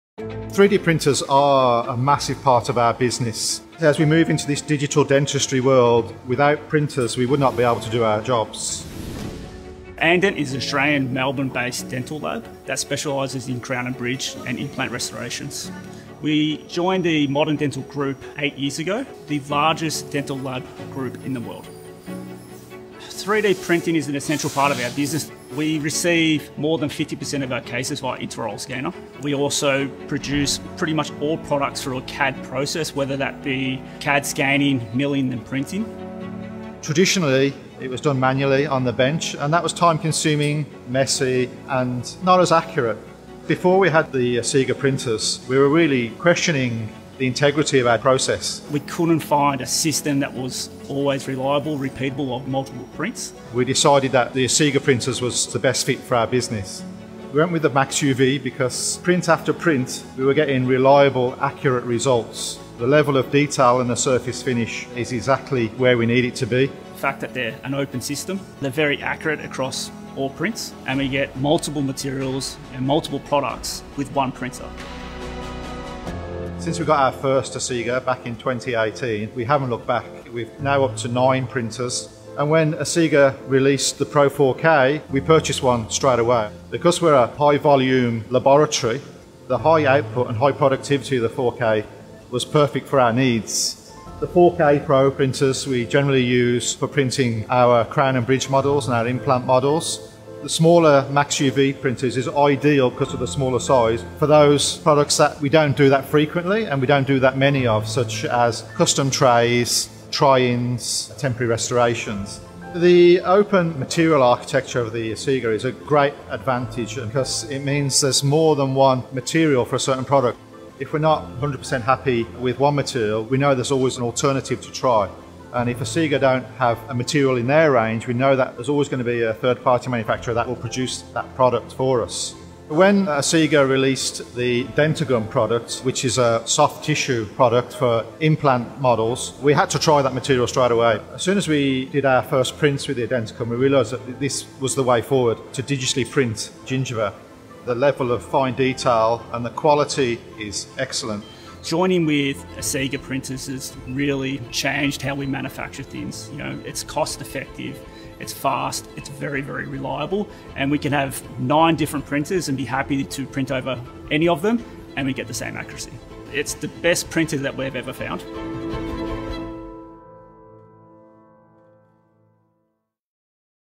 Asiga inside @andent aus, one of Australia’s largest dental laboratories
We were fortunate to gain access inside @andent aus, one of Australia’s largest dental laboratories